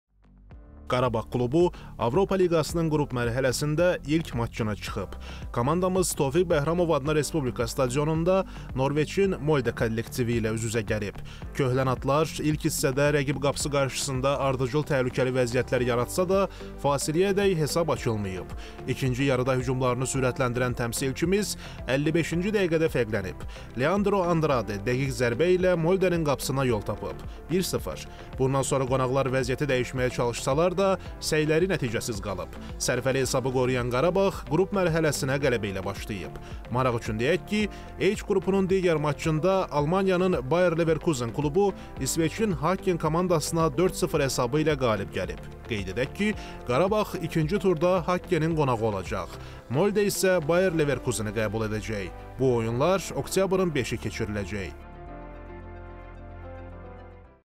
Avropa liqası: “Qarabağ” - “Molde” matçı
“Qarabağ” klubu Avropa liqasının qrup mərhələsində ilk matçına çıxıb.
Komandamız Tofiq Bəhramov adına Respublika stadionunda Norveçin “Molde” kollektivi ilə üz-üzə gəlib.
“Köhlən atlar” ilk hissədə rəqib qapısı qarşısında ardıcıl təhlükəli vəziyyətlər yaratsa da, fasiləyədək hesab açılmayıb. 2-ci yarıda hücumlarını sürətləndirən təmsilçimiz 55-ci dəqiqədə fərqlənib. Leandro Andrade dəqiq zərbə ilə “Molde”nin qapısına yol tapıb: 1-0.
Bundan sonra qonaqlar vəziyyəti dəyişməyə çalışsalar da, səyləri nəticəsiz qalıb. Sərfəli hesabı qoruyan “Qarabağ” qrup mərhələsinə qələbə ilə başlayıb.
Qeyd edək ki, “Qarabağ” 2-ci turda “Hakken”in qonağı olacaq. “Molde” isə “Bayer Leverkuzen”i qəbul edəcək. Bu oyunlar oktyabrın 5-i keçiriləcək.